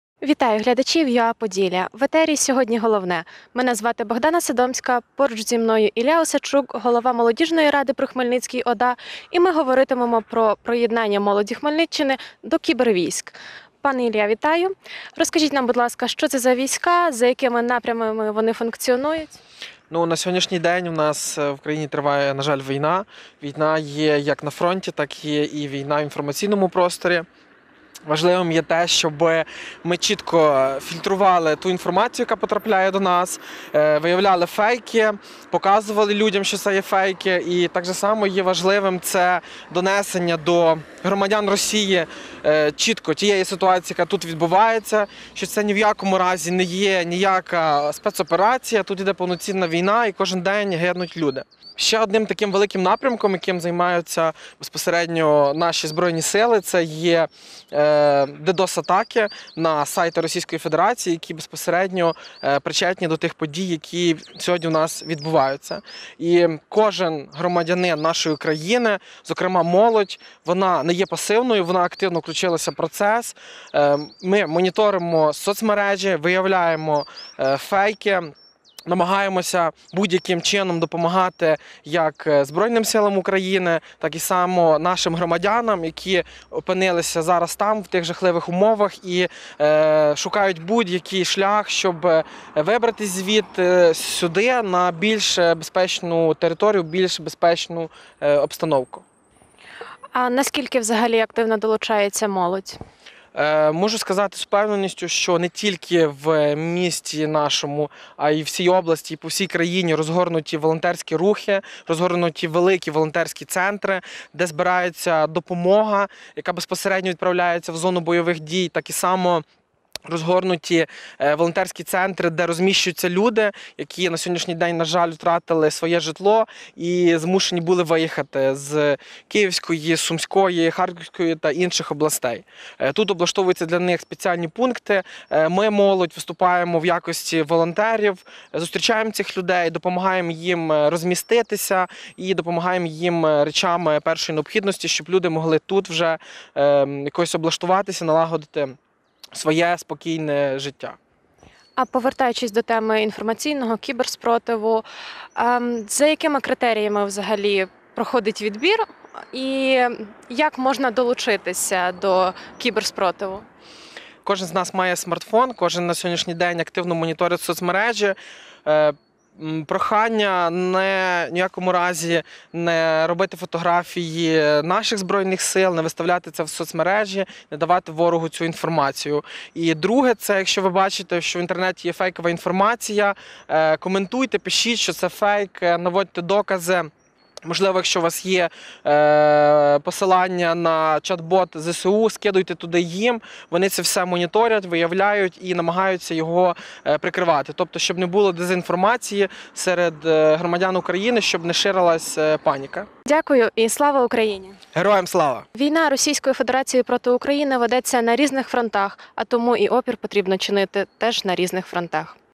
Як молодь Хмельниччини чинить опір в кіберпросторі
Ілля Осадчук, голова Молодіжної ради при Хмельницькій ОДА, розповів, як молодь Хмельниччини чинить опір в кіберпросторі. Ілля зазначив, що долучитись може кожен власник смартфона – для цього створений чат-бот @stop_russian_war_bot.